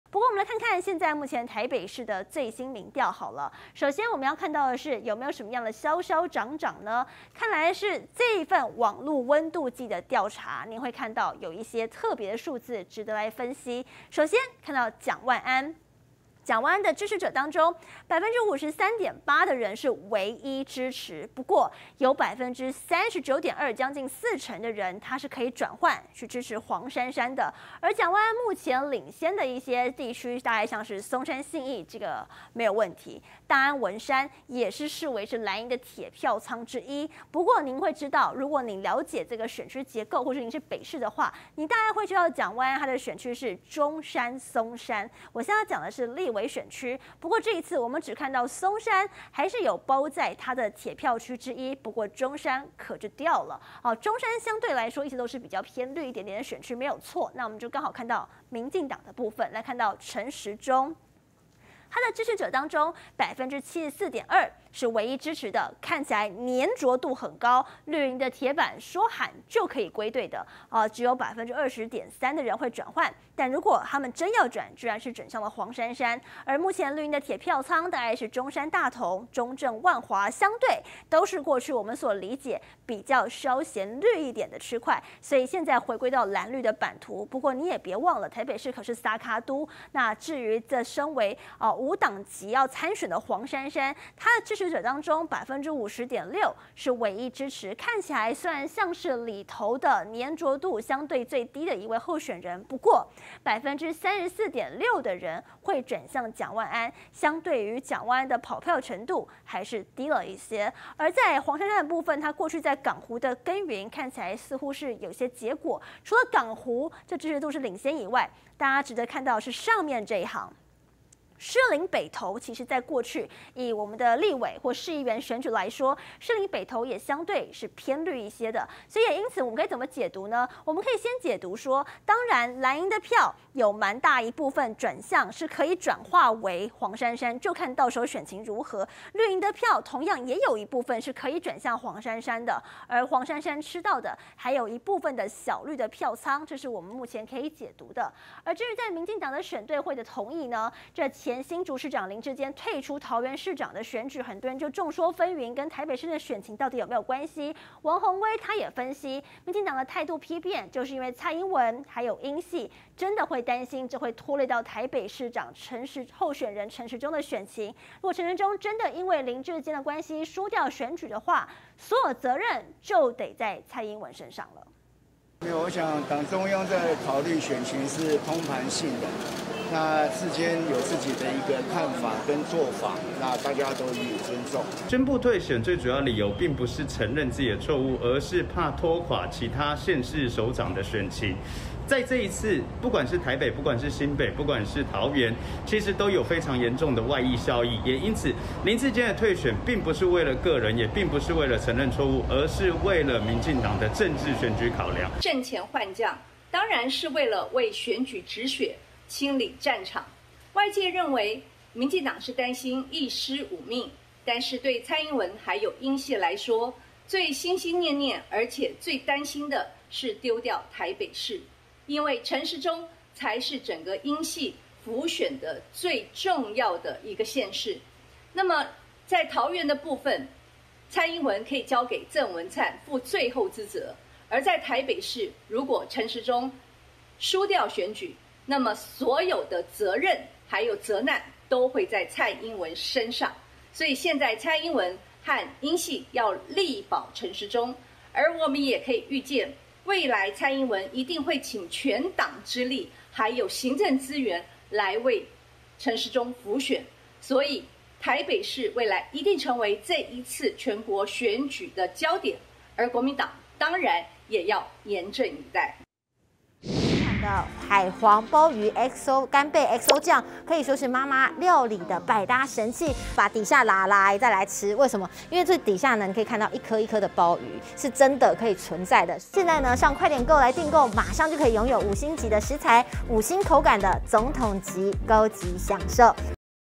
【每日必看】藍綠白戰北市! 民調:蔣萬安支持度28.3%領先｜民進黨態度轉彎關鍵!憂林智堅"拖累陳時中" ｜選舉戰略高地 20220813
00:00 藍綠白戰北市! 民調:蔣萬安支持度28.3%領先
02:29 民進黨態度轉彎關鍵!憂林智堅"拖累陳時中"